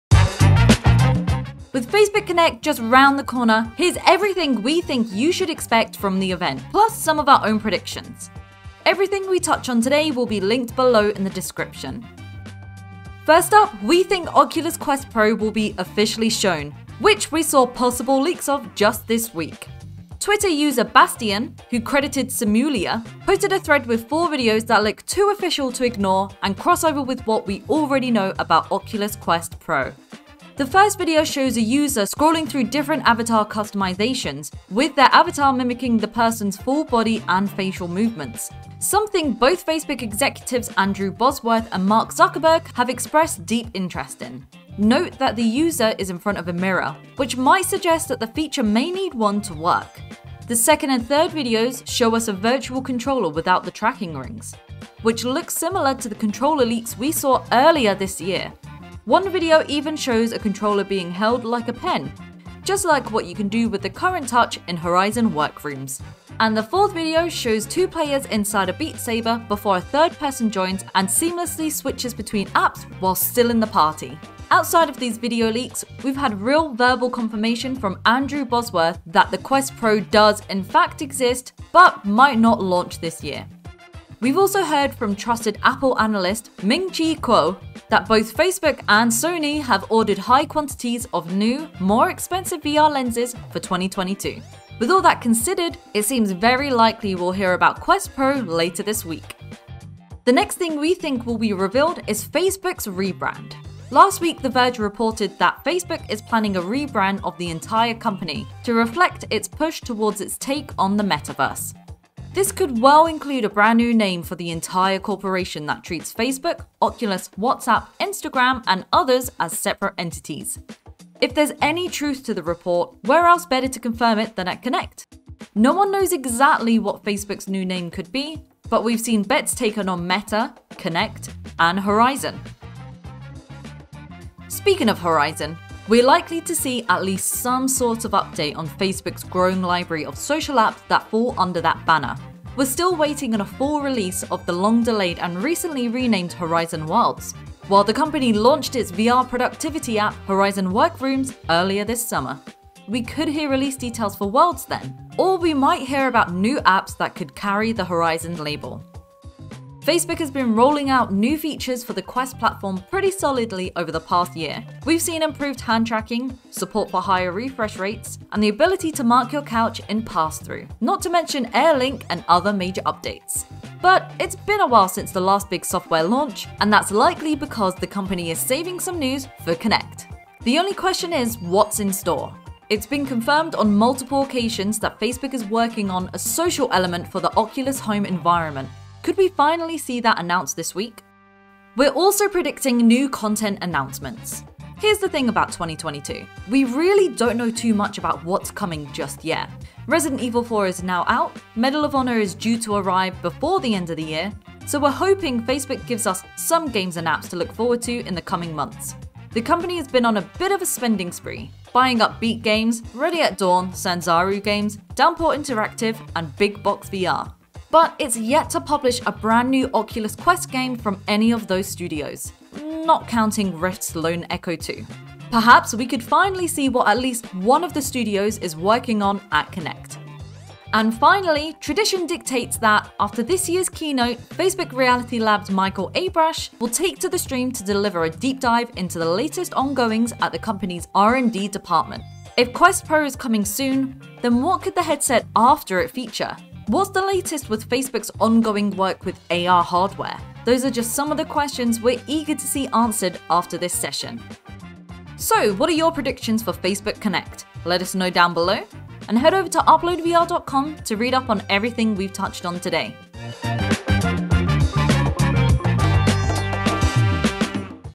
Oculus Quest Pro Leaks & Facebook Connect Predictions
Here's what we think will be announced at Facebook Connect 2021, and a summary of everything we know about Oculus Quest Pro

00:00 Intro
00:12 Oculus Quest Pro Leaks

01:40 Facebook Rebrand

02:14 Horizon Worlds Update

03:11 New Games & Apps (PLEASE!)
03:50 Michael Abrash Will Talk Future of VR